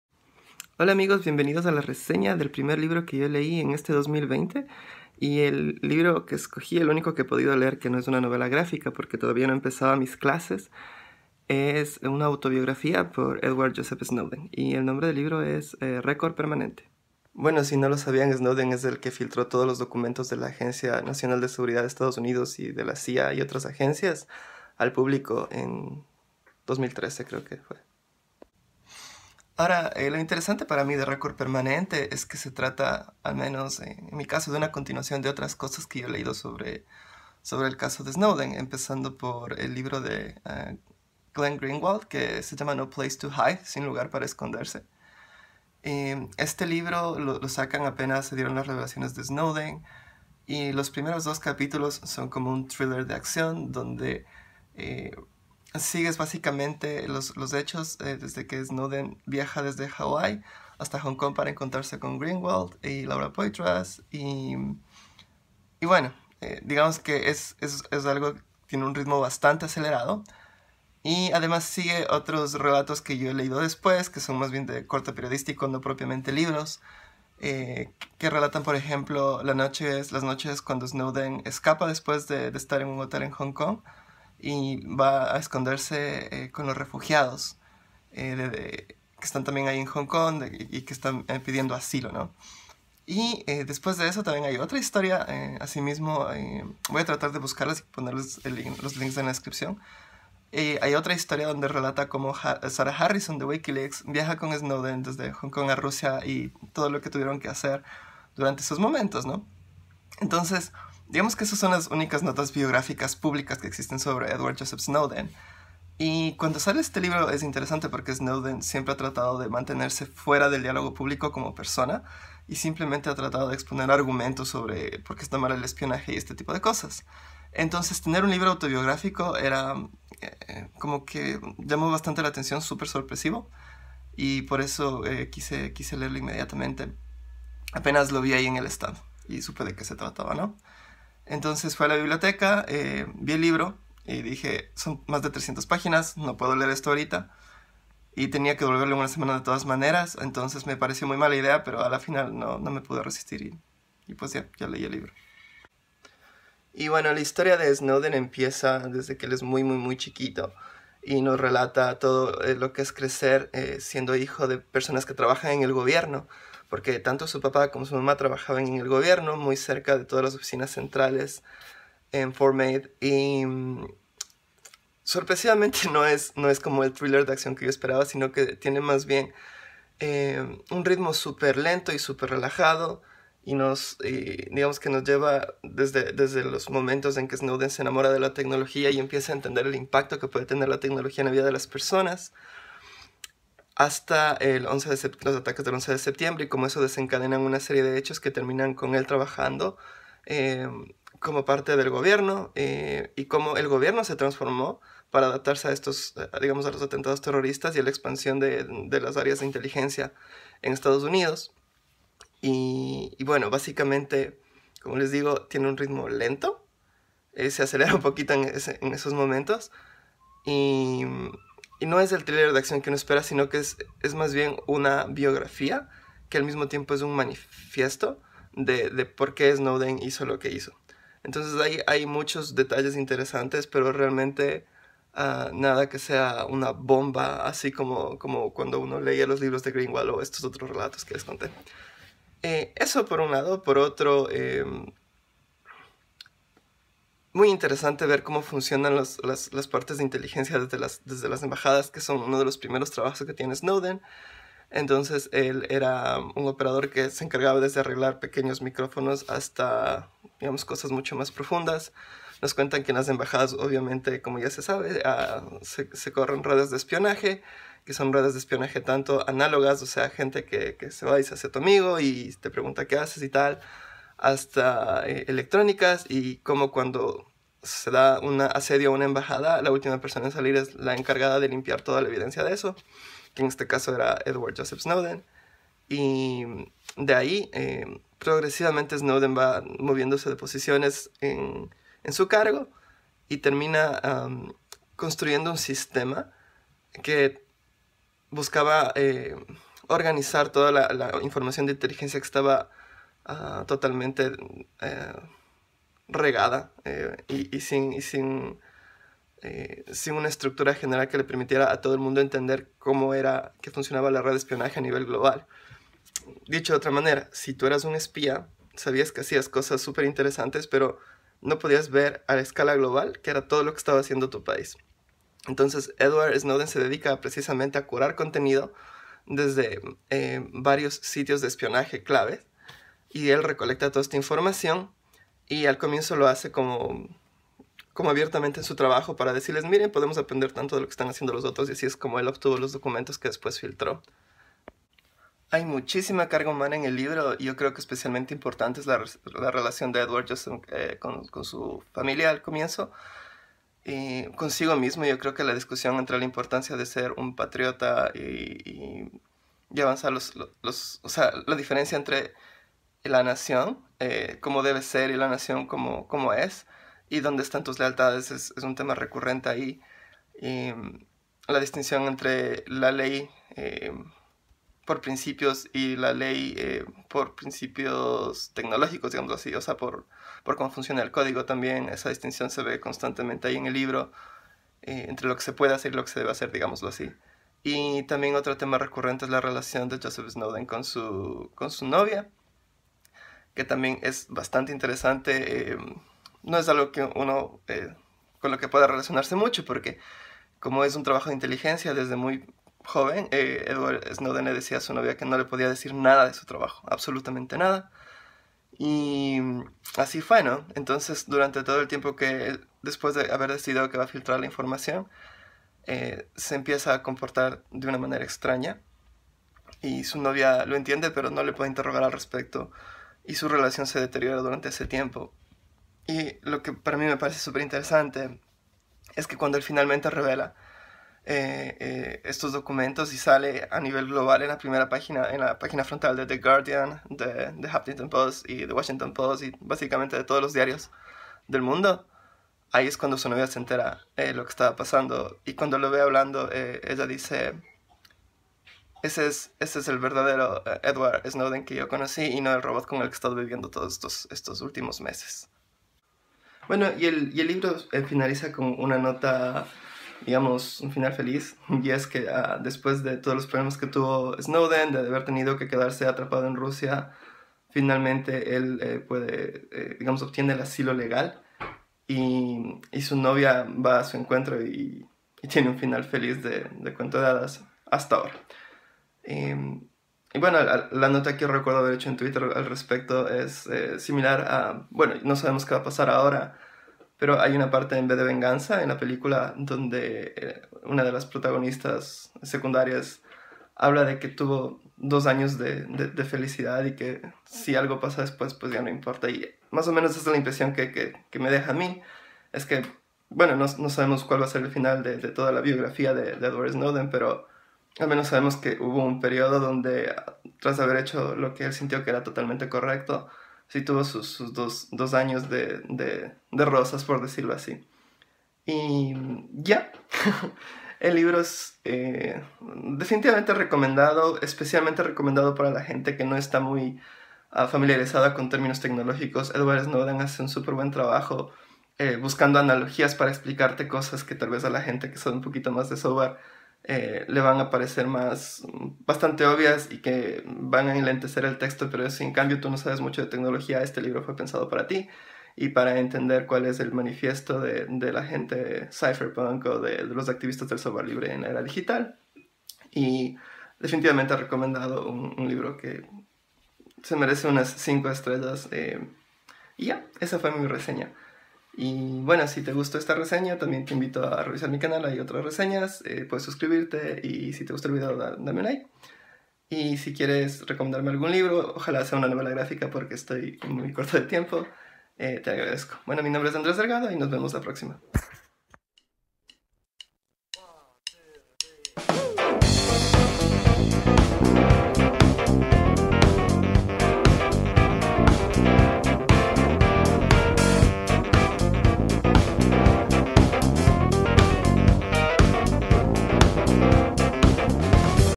Reseña en español: Record permanente - Edward Snowden (por
Reseña de la obra autobiográfica de Edward Snowden RECORD PERMANENTE. Aquí algunos textos que menciono en el video:

1. Sin un lugar donde esconderse
2. Perfil de Ed en Wired
3. Escape de Snowden en Hong Kong